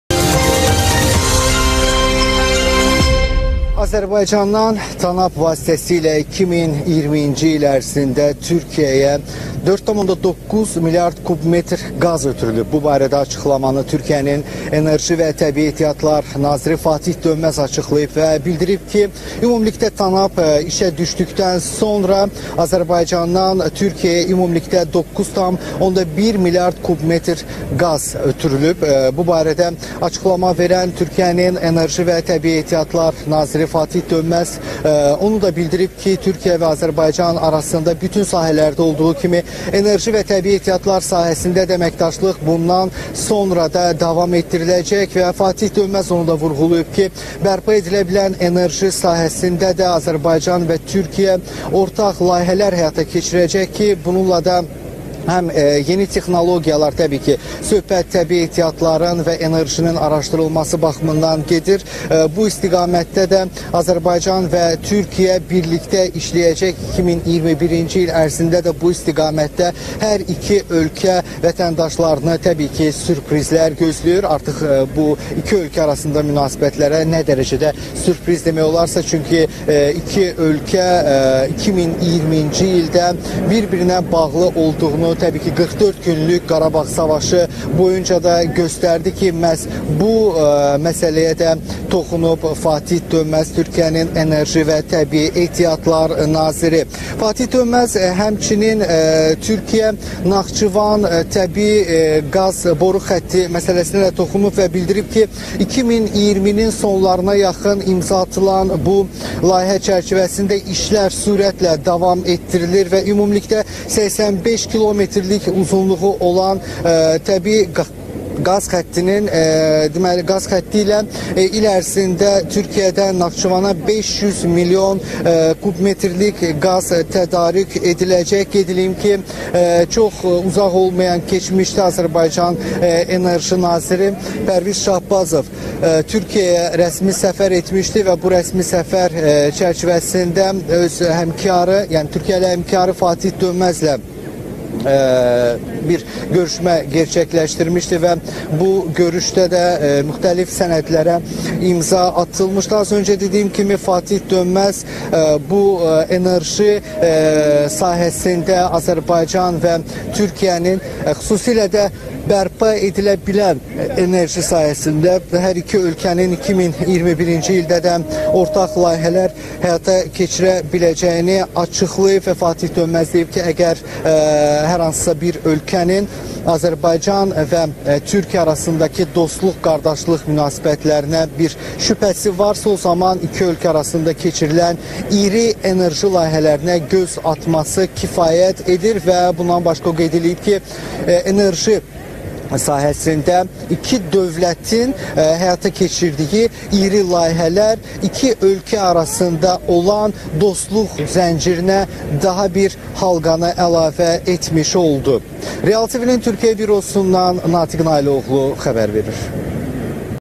Türkiyənin enerji nazirindən açıqlama
“TANAP”la ötürülən qazın həcmi 9,1 mlrd. kubmetrə çatdı